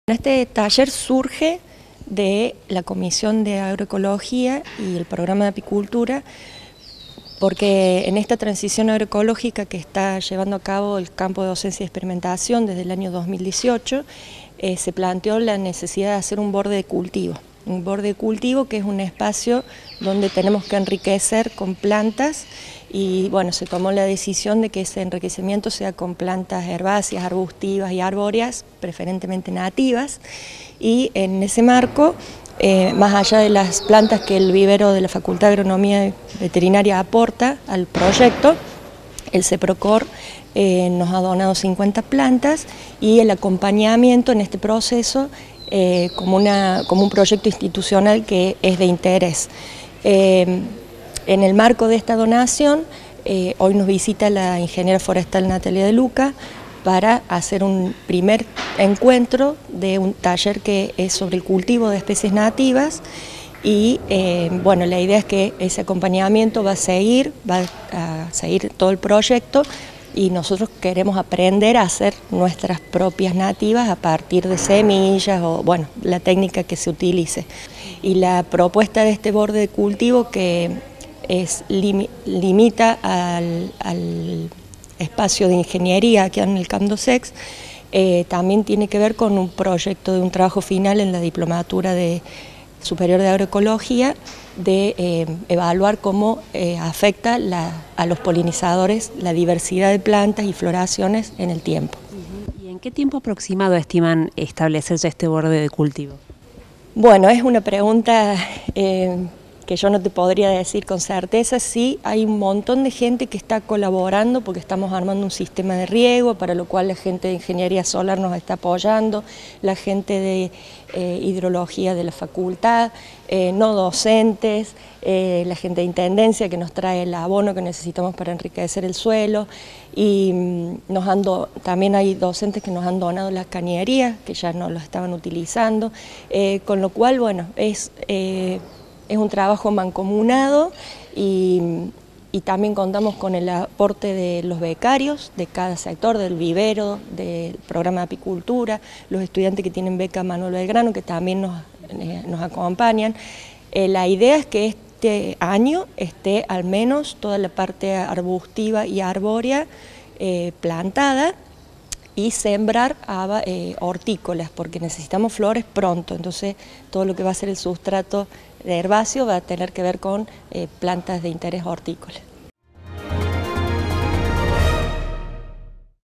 ESTUDIANTES Y PROFESIONALES PARTICIPARON DE UN TALLER SOBRE CULTIVO DE PLANTAS NATIVAS
PAULA MELEGATTI – INTEGRANTE PROGRAMA DE APICULTURA